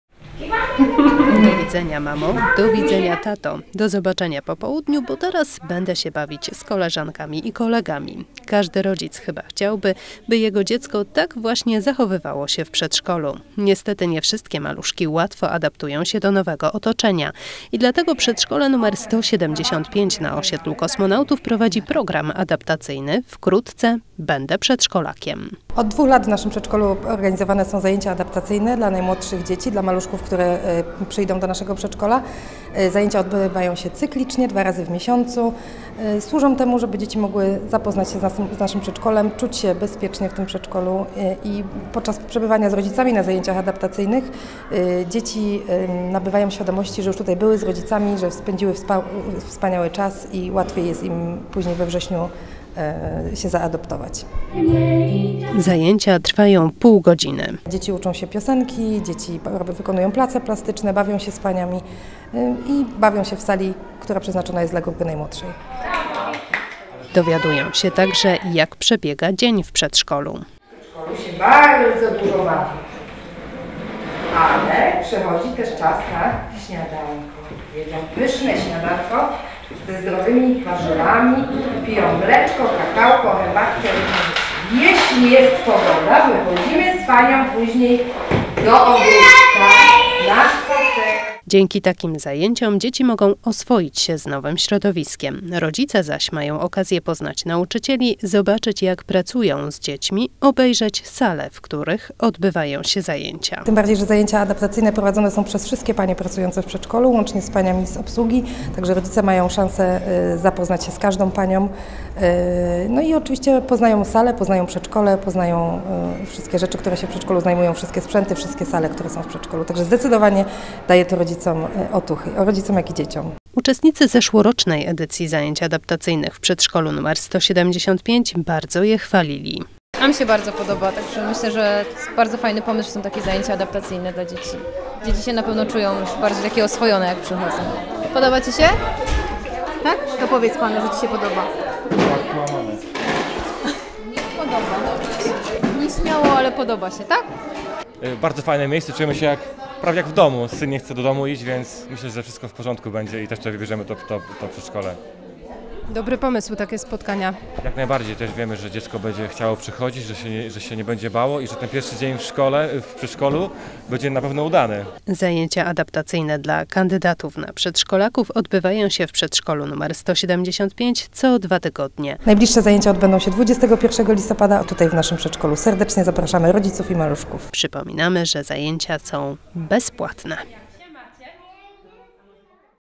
Przedszkole "Podniebny świat" zaprasza na zajęcia adaptacyjne
Pierwsze dni w przedszkolu – to dla dzieci wielkie przeżycie. Jak sprawić, by dziecko nie bało się przedszkola i by chciało do niego chodzić? Mogą w tym pomóc zajęcia adaptacyjne. Takie zajęcia proponuje Przedszkole nr 175 na os. Kosmonautów.